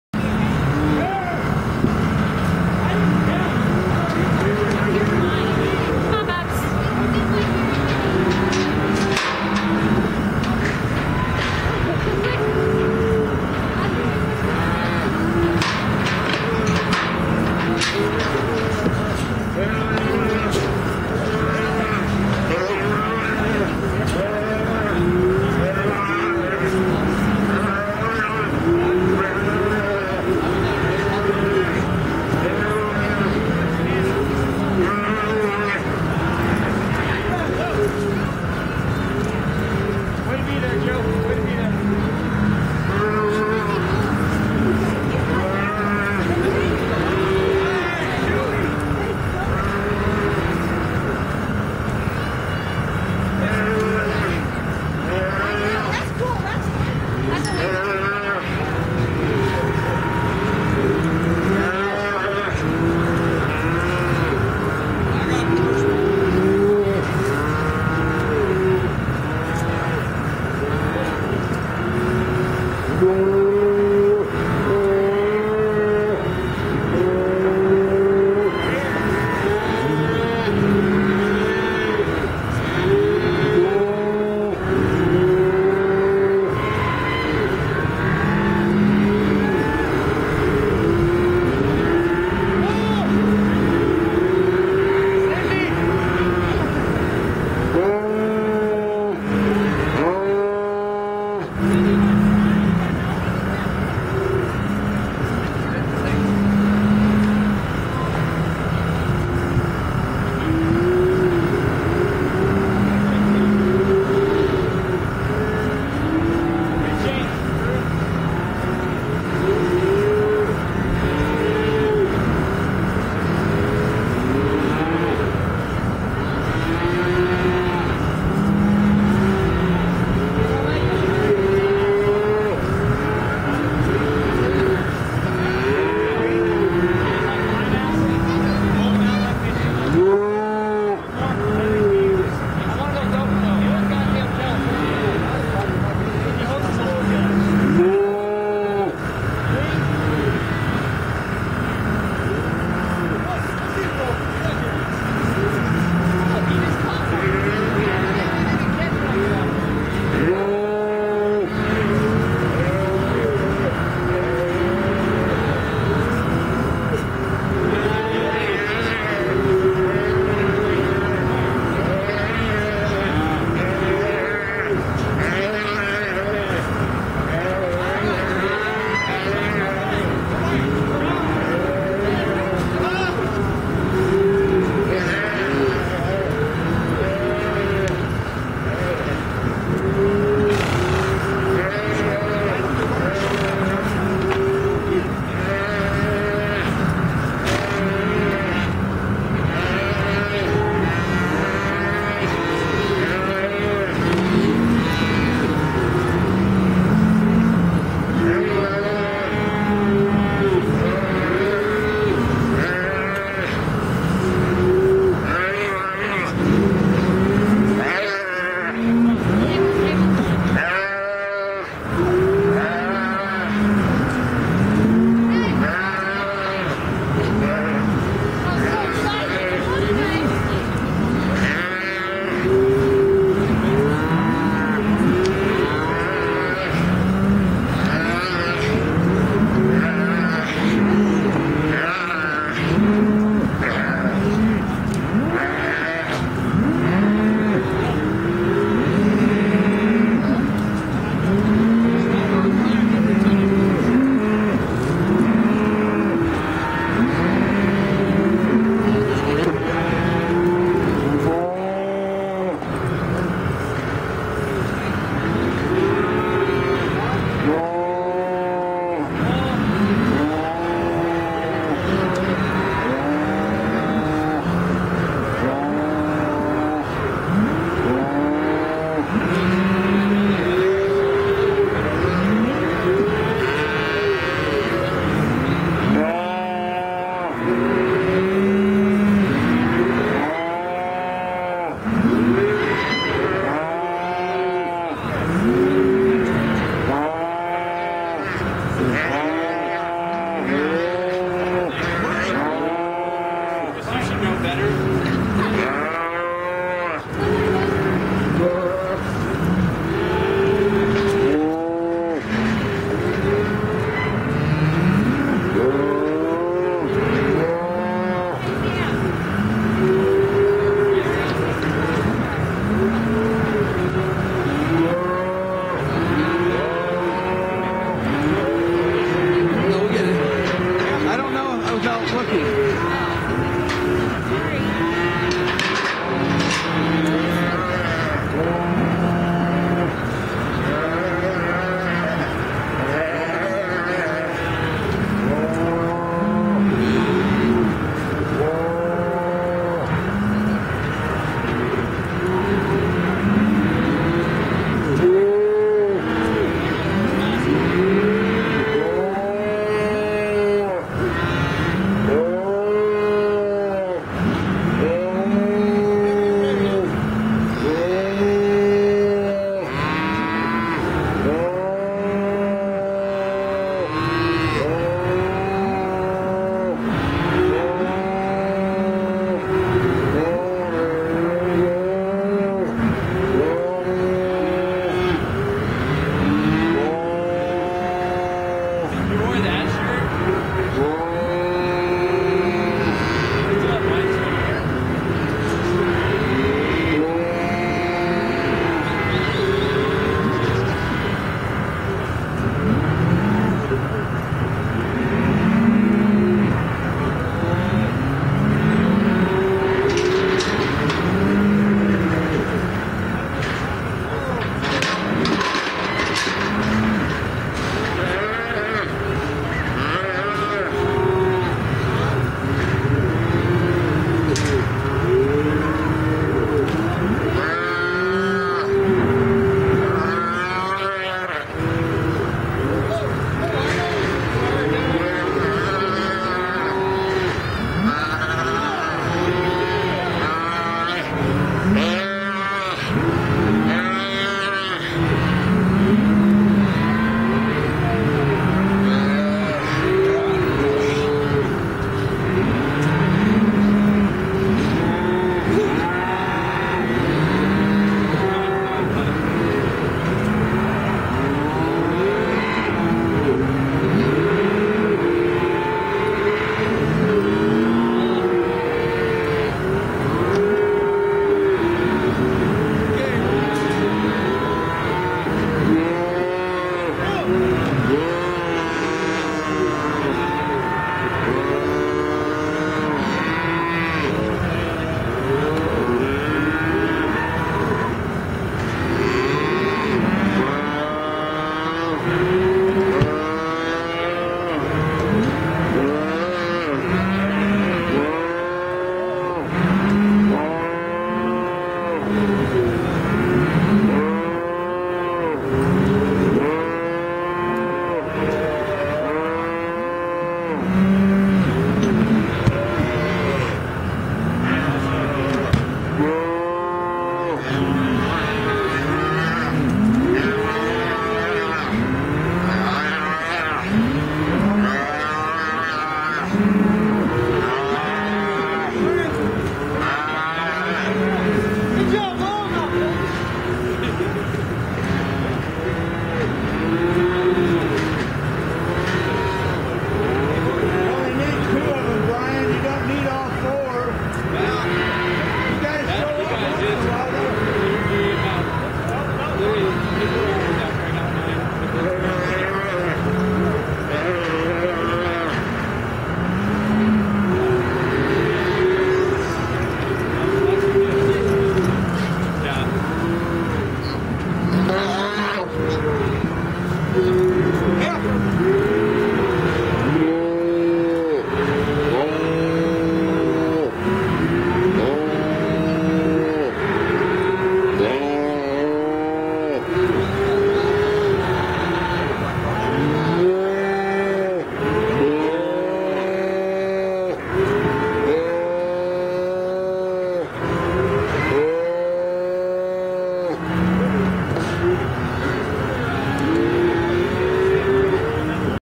2009 AQHA Sorrel Mare by Pabst Budha Ribbon
FOR SALE: Grey three year old AQHA gelding and sorrel three year old AQHA filly in calf branding pen. Both for sale.  Sorrel filly's rider has straw hat and blue coat. The grey colt's dam is the dark sorrel mare with a snip on her nose.